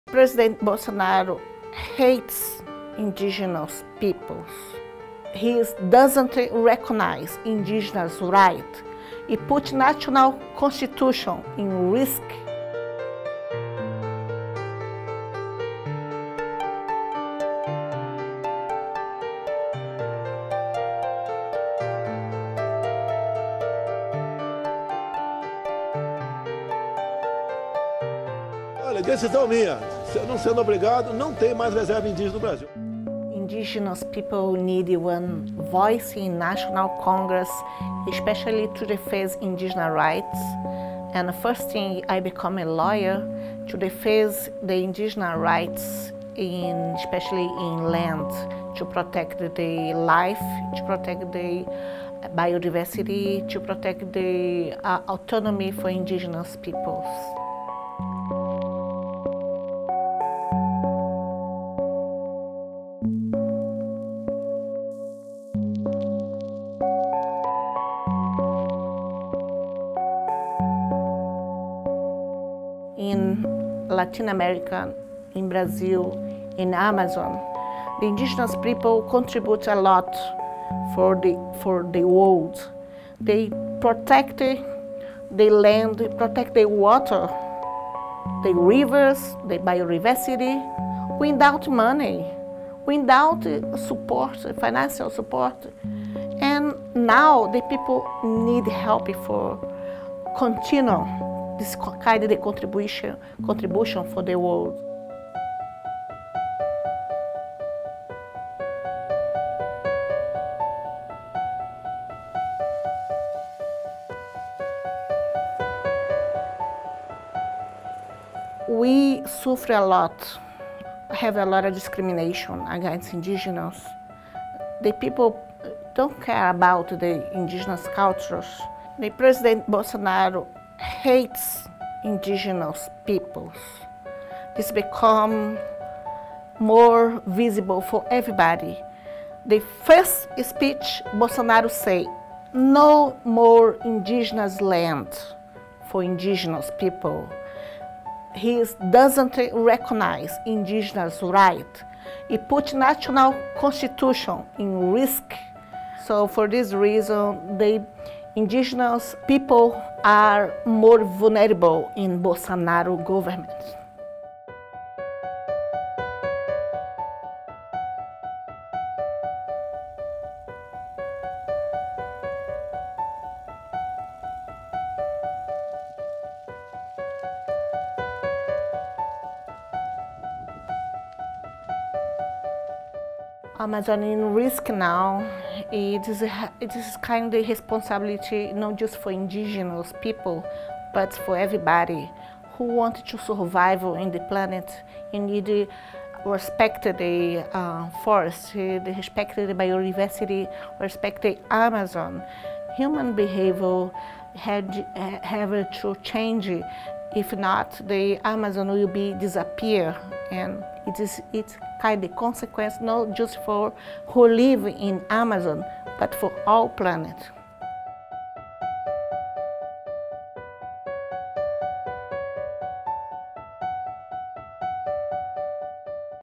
How Brazilian President Jair Bolsonaro Puts Indigenous People at Risk | NowThis
Brazil's first Indigenous congresswoman, Joênia Wapixana, is leading the fight to protect the Amazon forest from far-right President Jair Bolsonaro.

In US news and current events today, meet Joênia Wapixana, Brazil's first Indigenous congresswoman and activist.

Wapixana has vowed to fight for Brazil’s indigenous people and their land, which Brazilian President Jair Bolsonaro has threatened to take away. The far-right Brazilian president has vowed to open up protected native lands to economic and commercial development, which has encouraged miners, loggers, and farmers to invade native lands.

Deforestation in the Amazon surged 30% btwn Aug 2018 and July 2019 and 2019 saw an 80% increase in fires, more than half of which were in the Amazon. Experts attribute this to weakened protections under Bolsonaro and illegal deforestation.

Watch our full interview with Amazon and Indigenous activist Joênia Wapixana here.